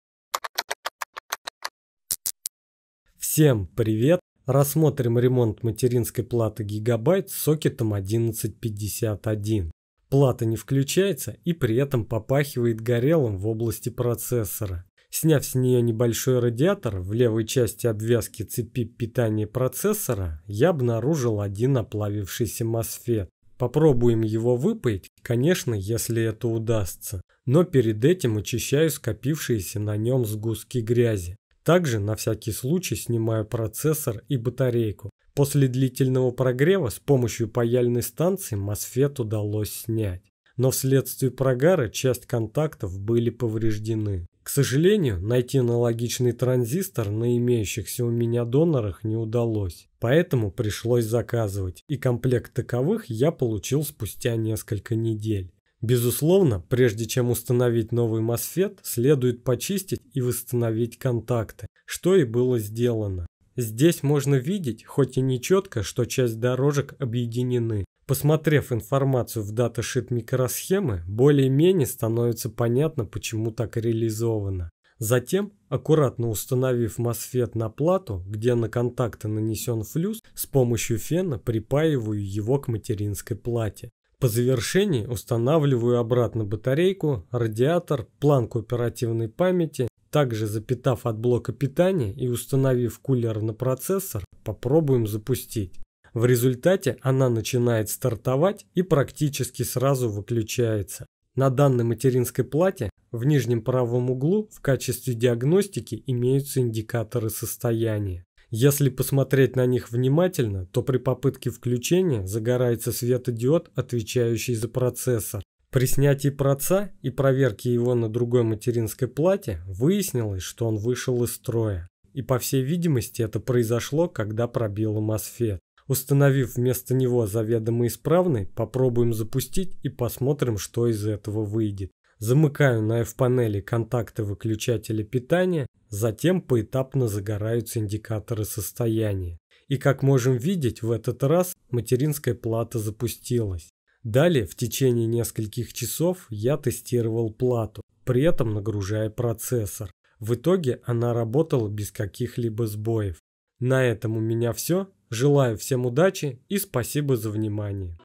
Ремонт материнской платы. Не включается, пахнет горелым
Рассмотрим ремонт материнской платы GIGABYTE GA-Z270XP-SLI (rev. 1.0) с сокетом 1151 (стоявшей на риге). Плата не включается, и при этом попахивает горелым в области процессора.